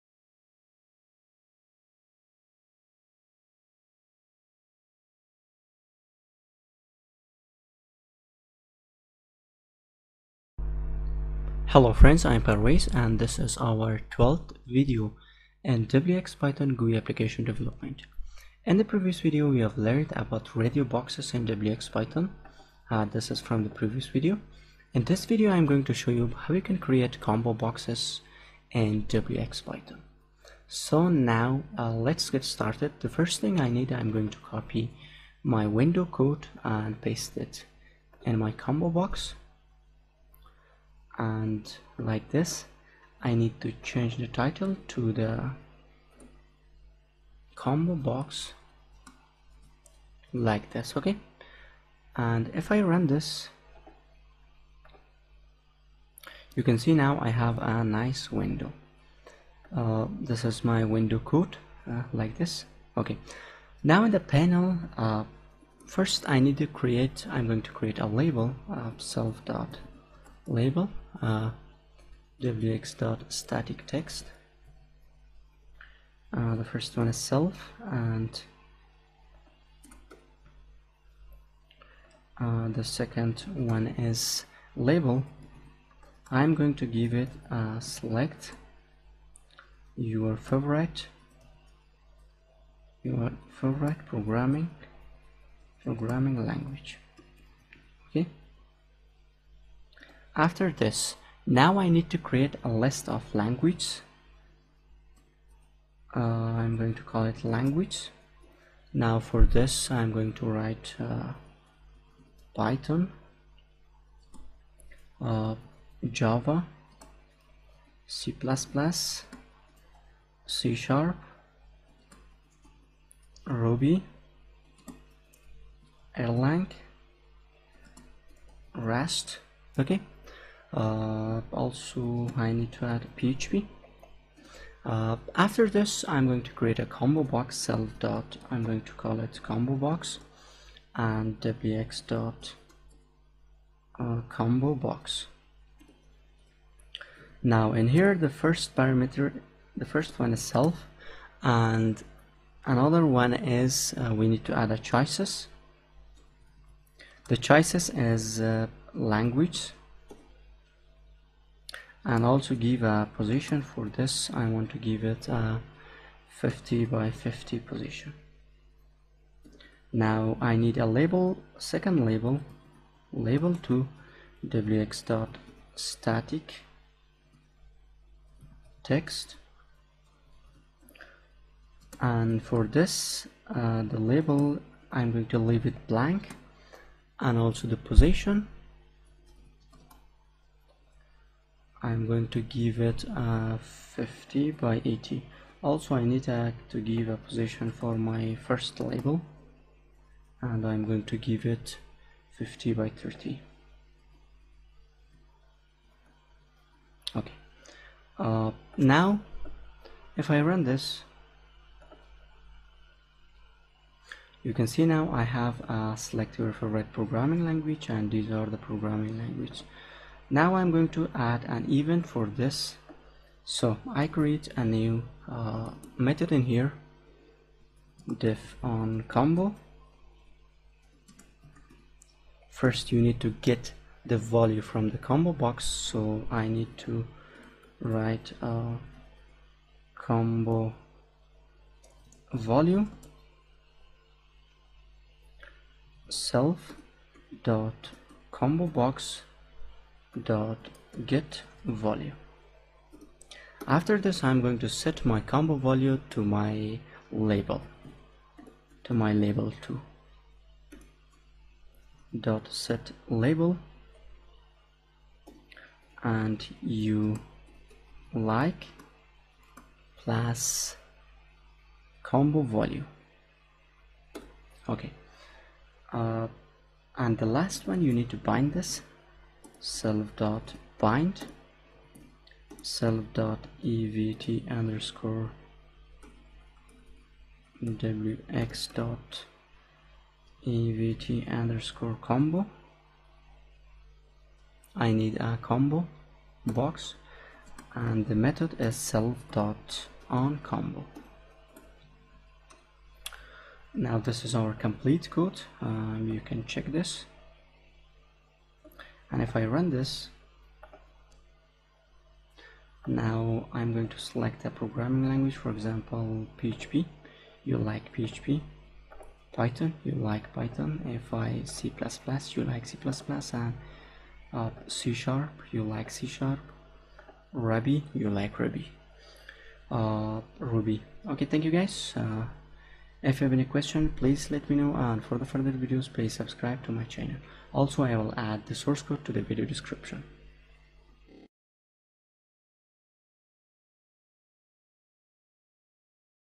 Create ComboBox In wxPython #12
In this video iam going to show you how to Create ComboBox In wxPython Creating ComboBox In wxPython is so easy and simple